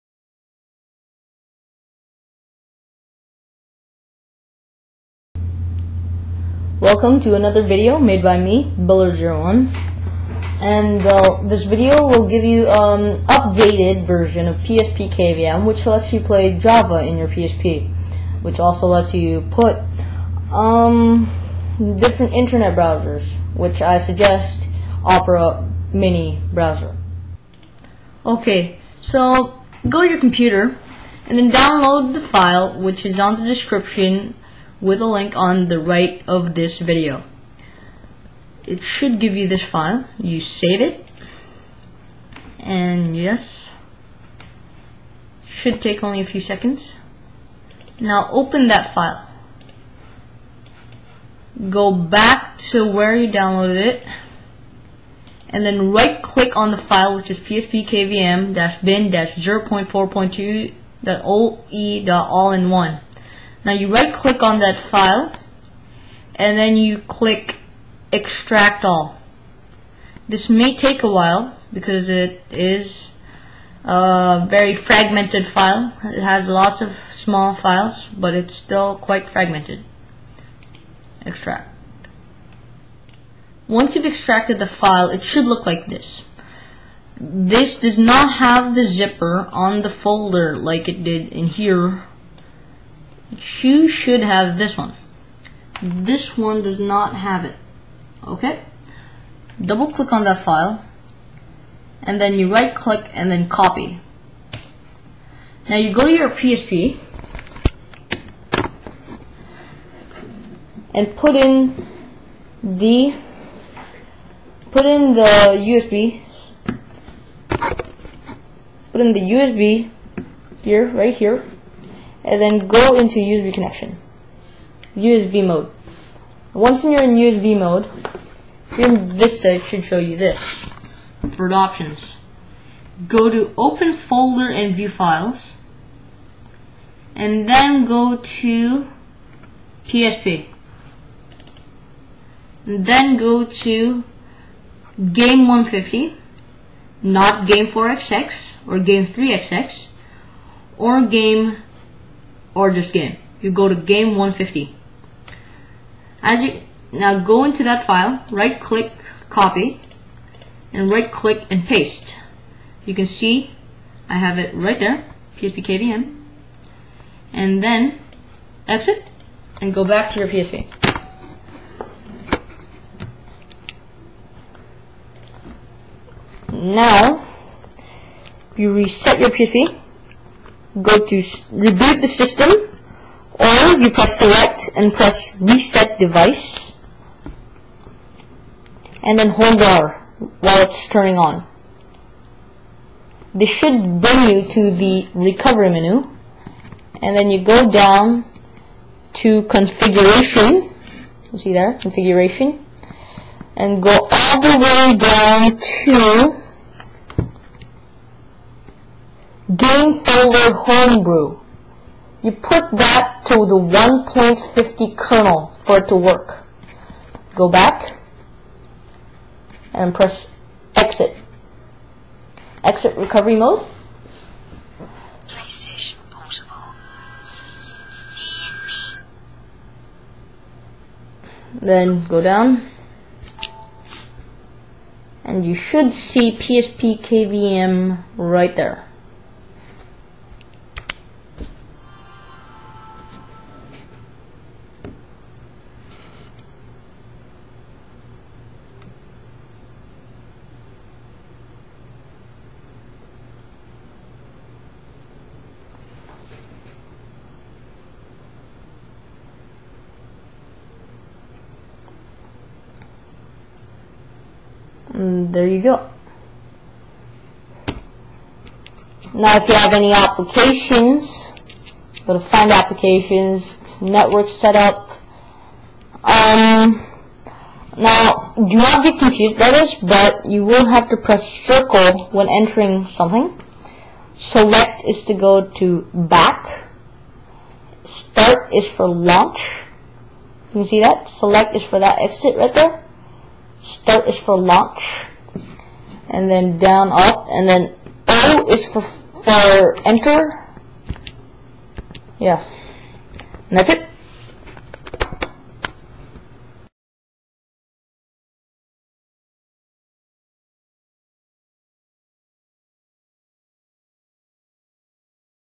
PSPkvm 0.4.2 (JAVA) for PSP
This will show you the updated video on how to put the updated java player on to your psp